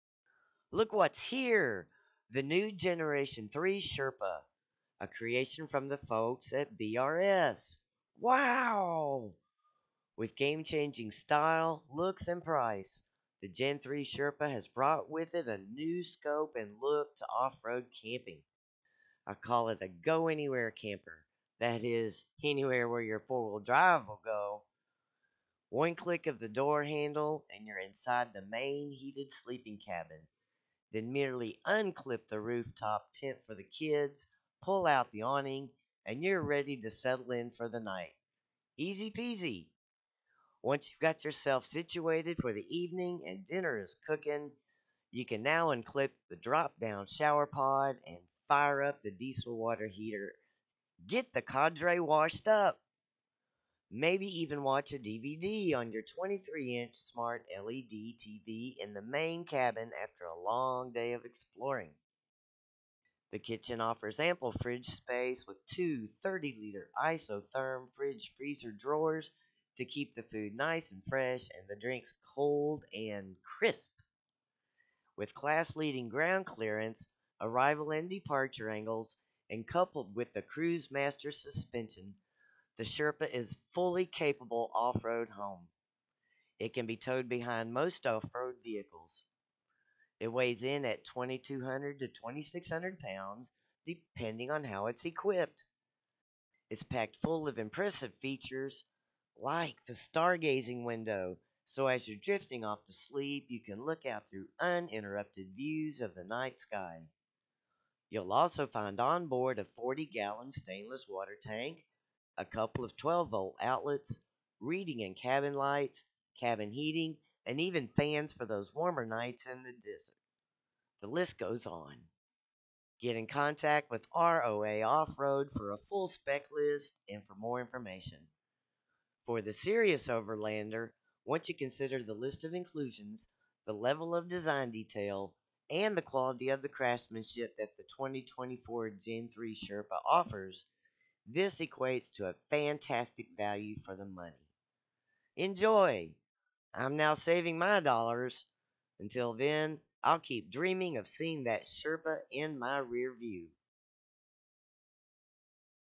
First Look! The BRS SHERPA GEN 3! ROA OFF ROAD!
For a while now, I've been considering an off-road capable trailer for my future preparedness adventures.  We all know overlanding and preparedness go hand-in-hand.
The BRS Sherpa has been a top contender as my choice for an off-road home.
Bugging out?  Do it in a Sherpa. Live large in a small place!  Go anywhere!
Today, I came across this raw information on the 2024 Gen 3.
Hope you enjoy this as much as I did.
The sherpa is guaranteed to be a monster of an off-road home in any SHTF scenario.

For more information on the 2024 Gen 3 Sherpa, visit RVs of America